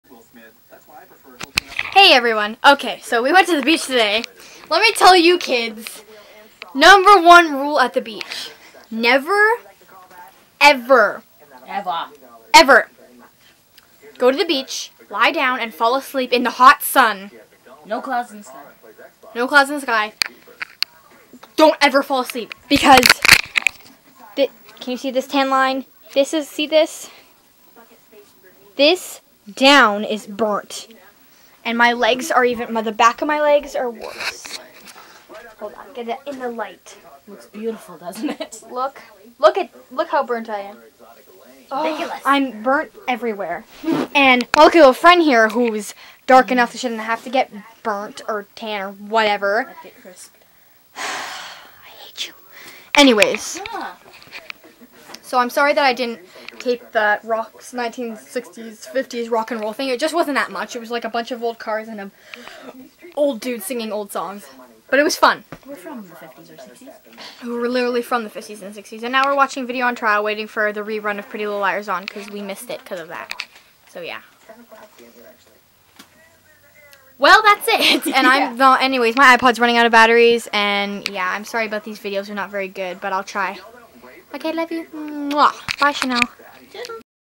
Sauble Beach Part 3: Sun Burnt!
Falling asleep on the beach. BAD IDEA!